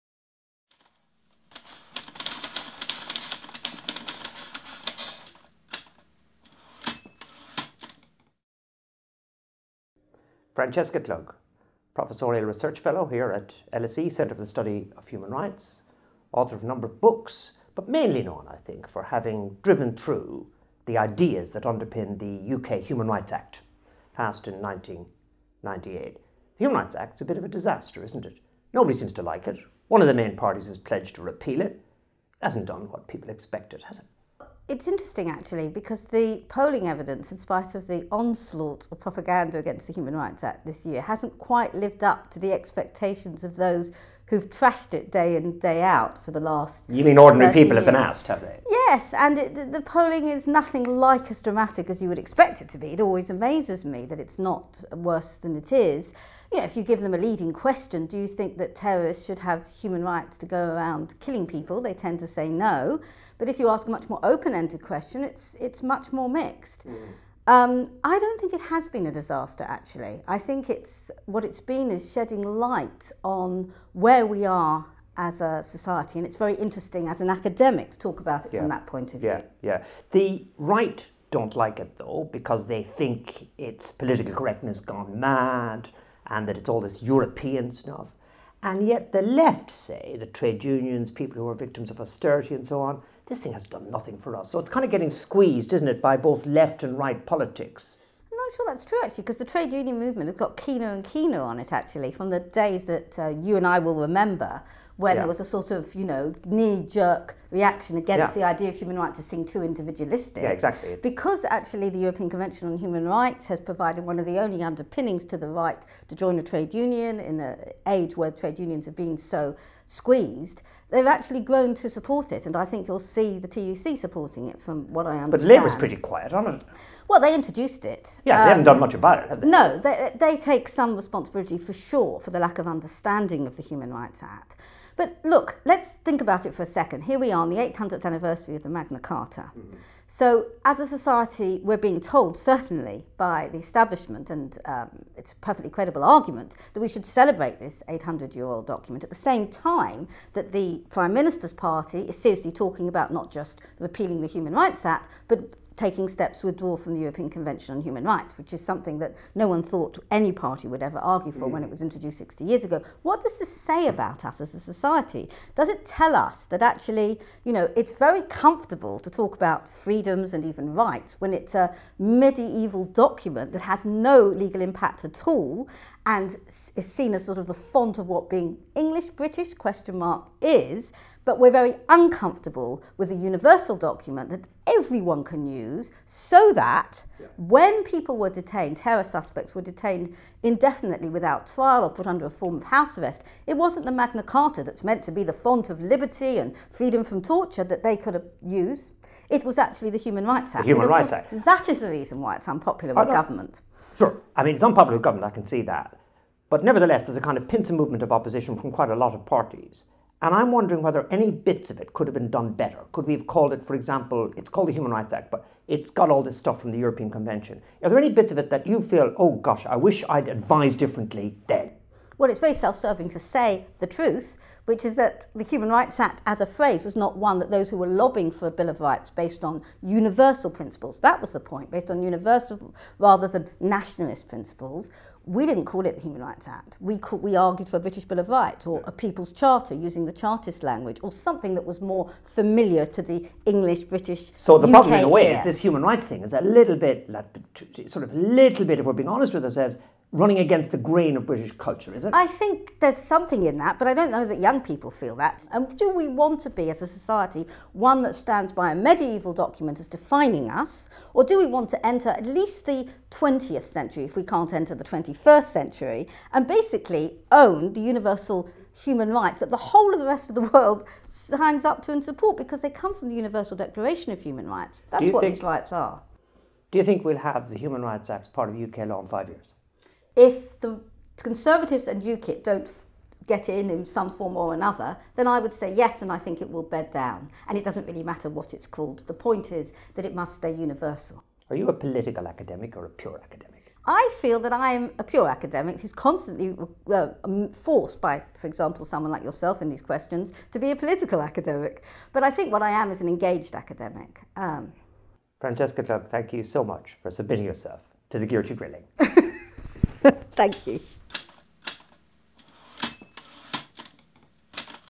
Gearty Grilling: Francesca Klug on Human Rights
Francesca Klug, Professorial Research Fellow, discusses human rights and the Magna Carta.